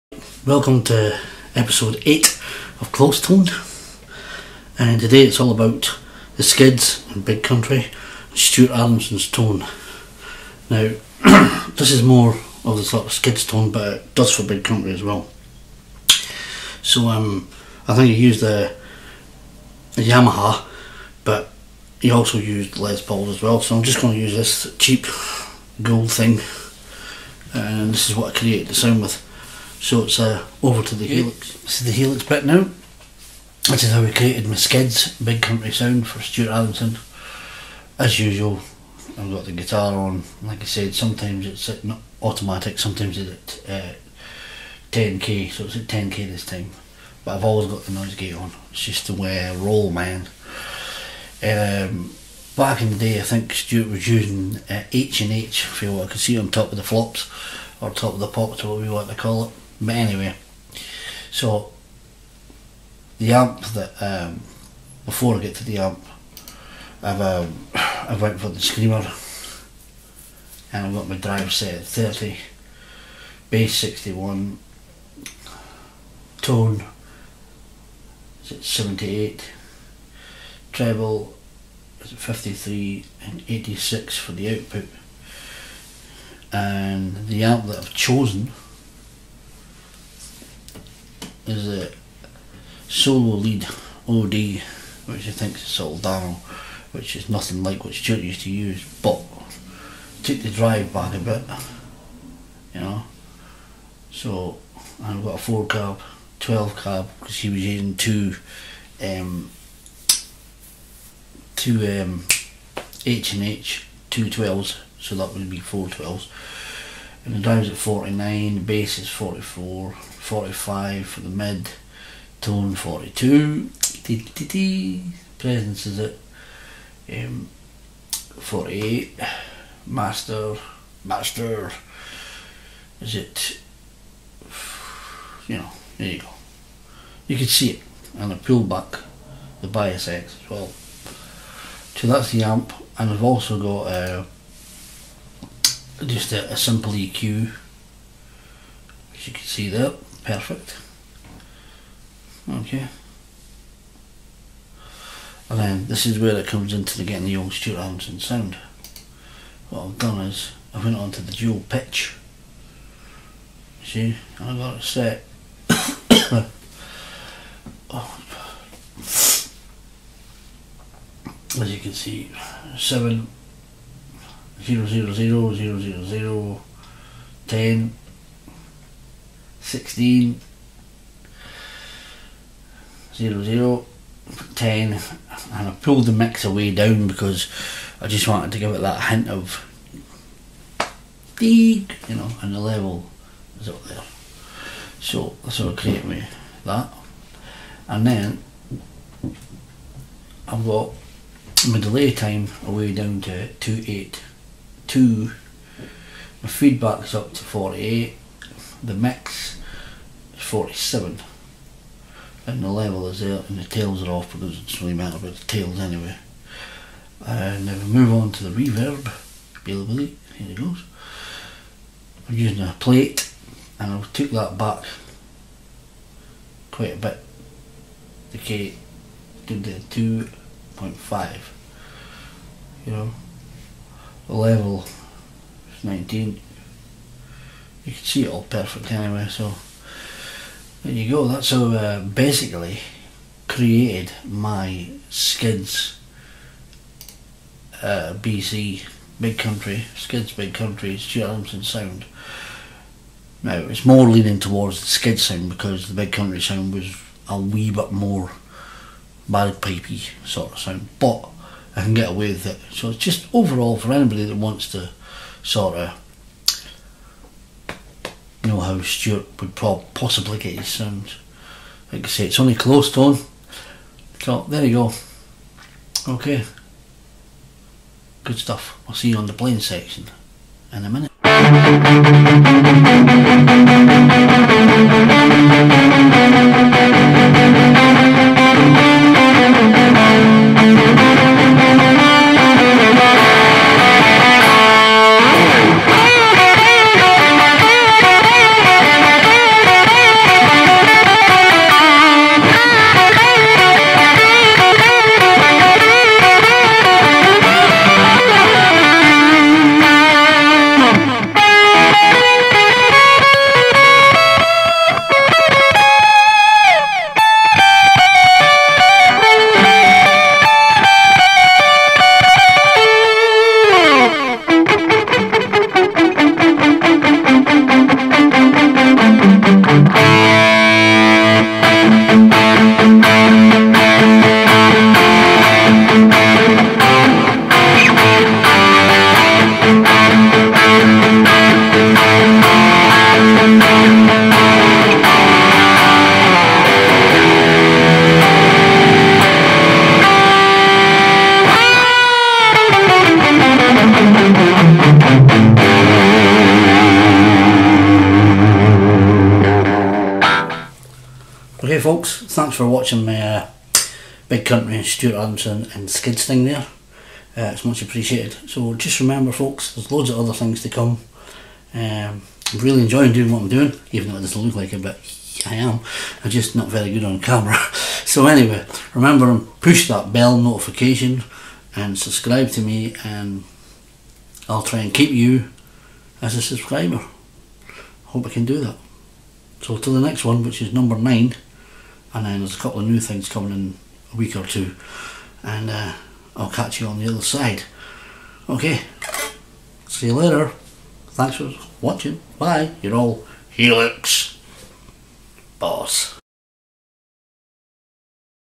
HELIX EP 8 CLOSE TONE. STUART ADAMSON / SKIDS. BIG COUNTRY
TRYING TO GET A CLOSE TONE TO STUART,S TONE